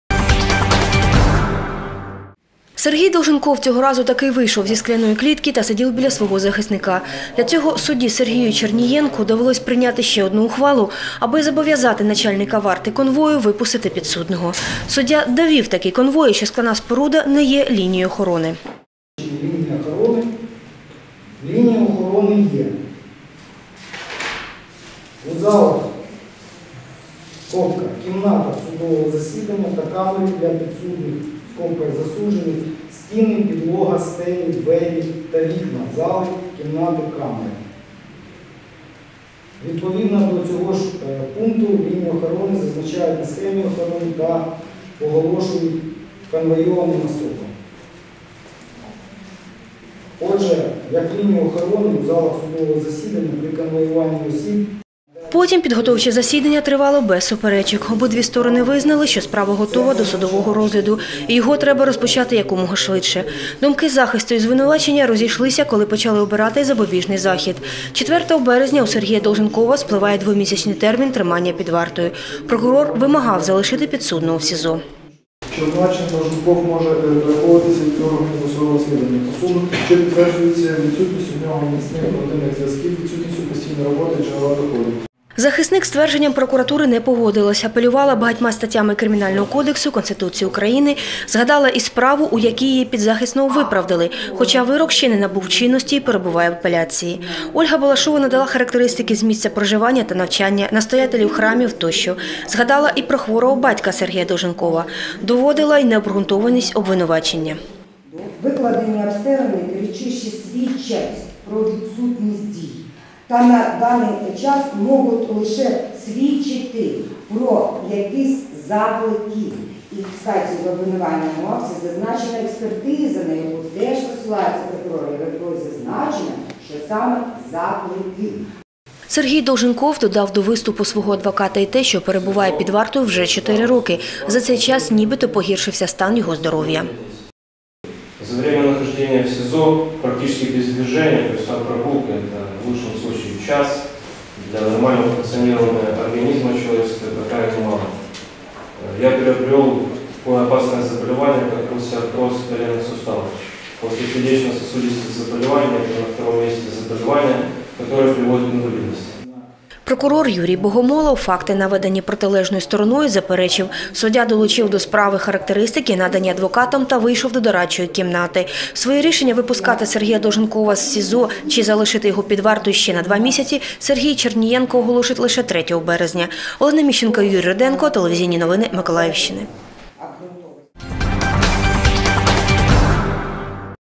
Суд над антимайданівцем
У Центральному районному суді триває підготовче засідання у справі антимайданівця Сергія Долженкова. Цього разу учасники процесу таки визначились, чи можна розпочинати розгляд по суті, та обирали запобіжний захід підсудному.  А от чи обрали – знає Олена Міщенко.